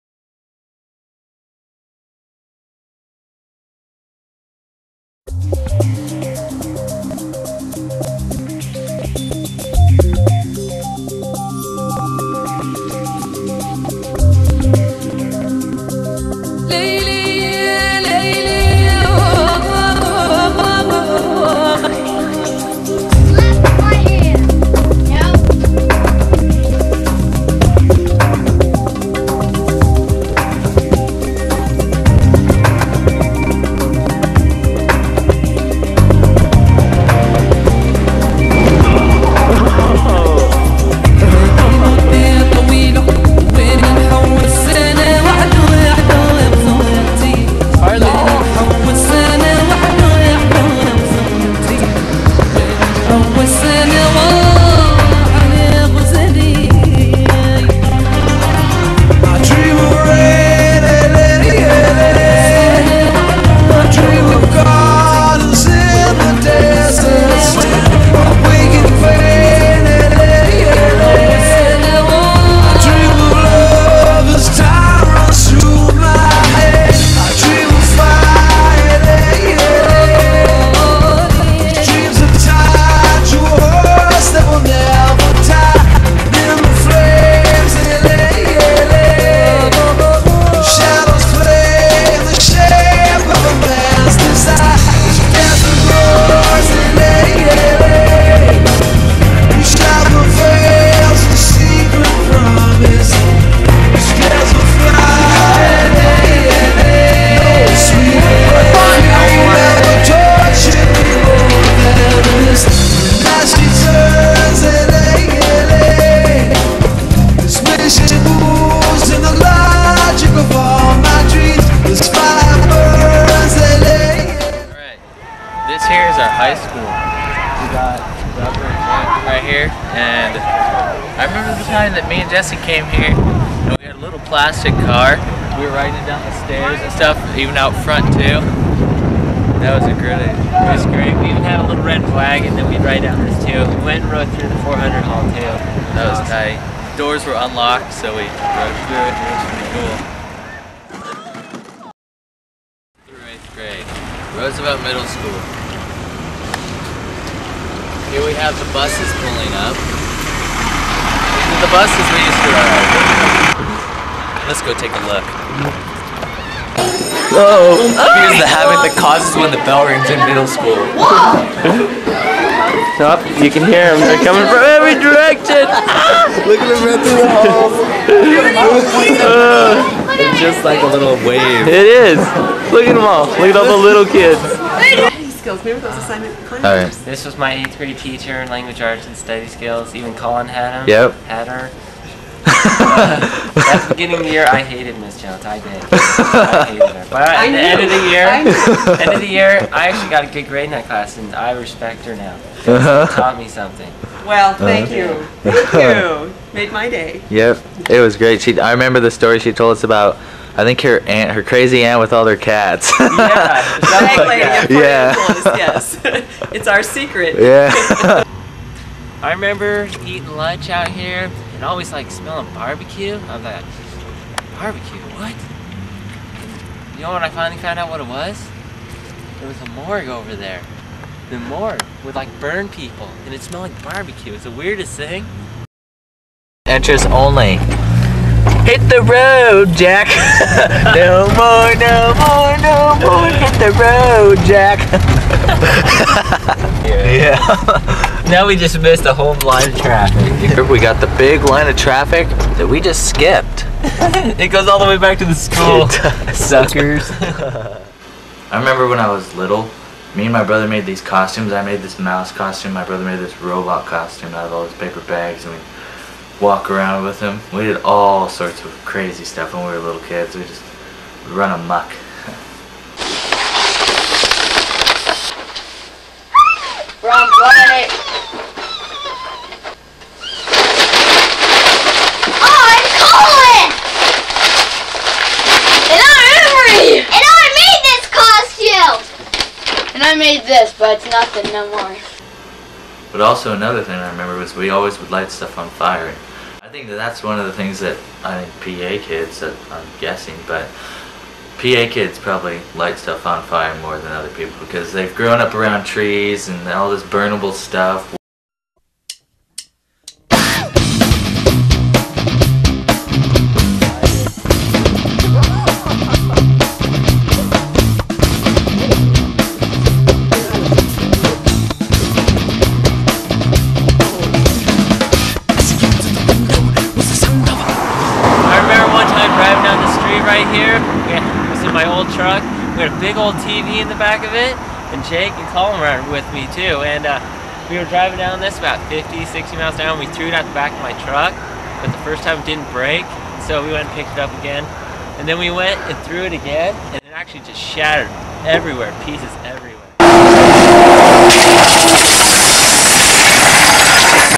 Growing Up in Port Angeles Part 1 of 3
A short documentary of my friends and I growing up in Port Angeles. The footage dates from 1989 to 2002. As soon as Jackass came out filming slowed down.  I edited this together for my 12th grade Senior Project in 2002. It was my first time using a computer with editing software (Video Wave).